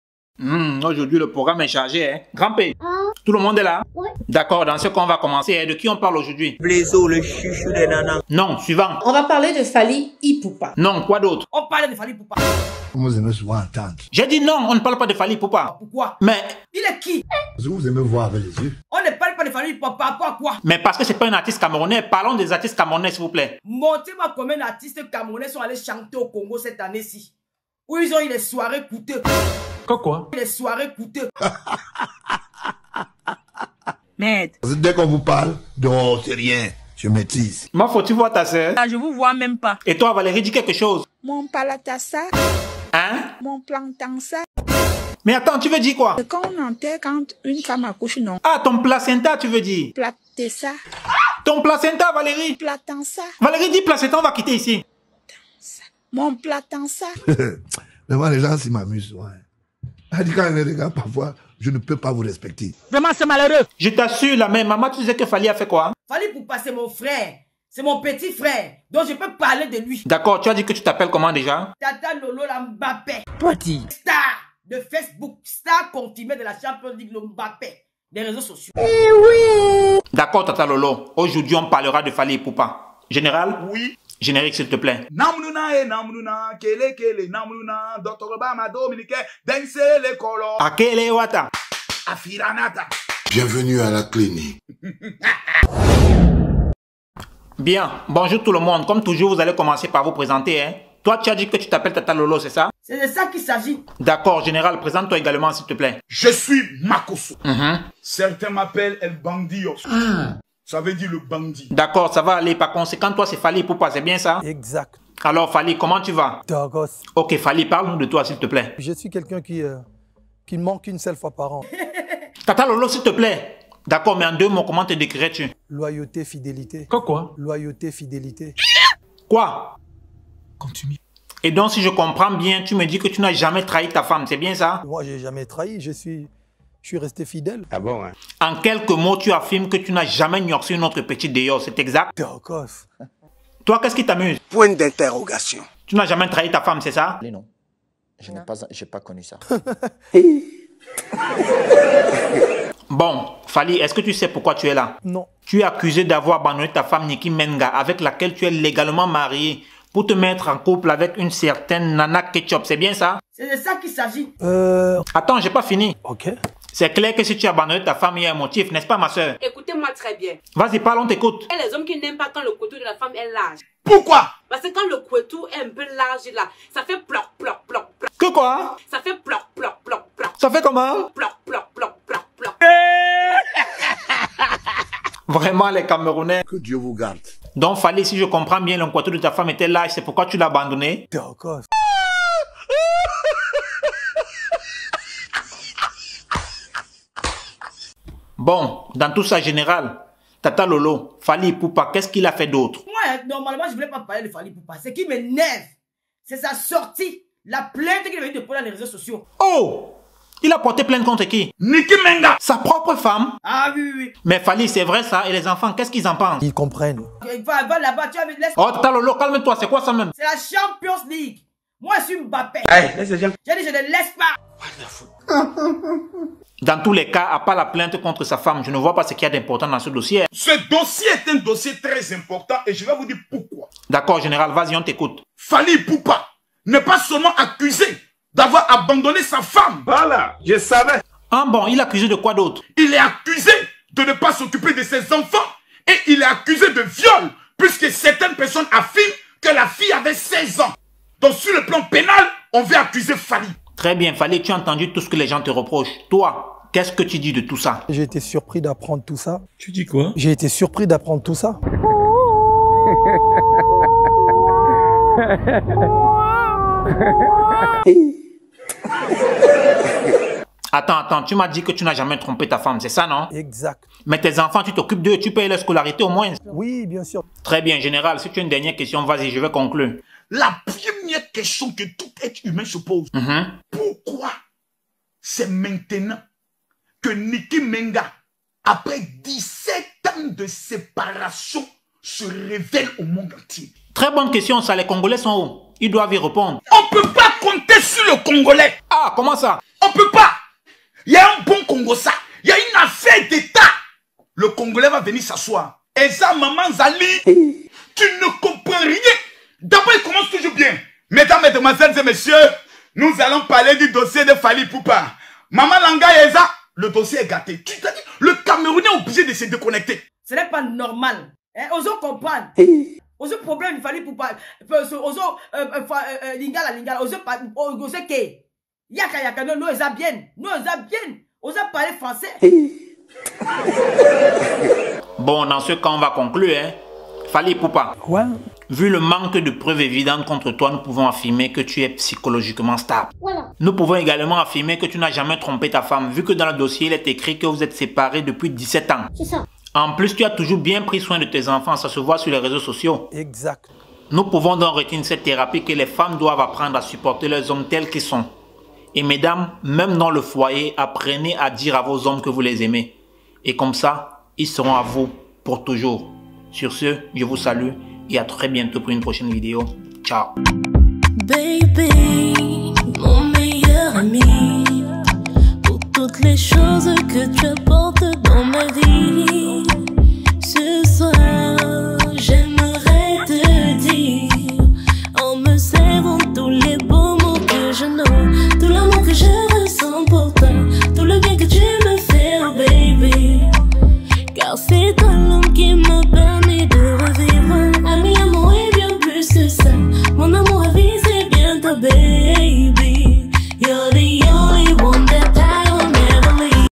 MAKOSSO FAIT DES RÉVÉLATIONS SUR FALLY IPUPA
Révélations sur le dossier fally ipupa l'histoire de sa femme ex femme ,la plainte,l'arrestation des blogueurs congolais,sa vie sécrete dévoilée.accrocher vous vous n'en reviendrez pas.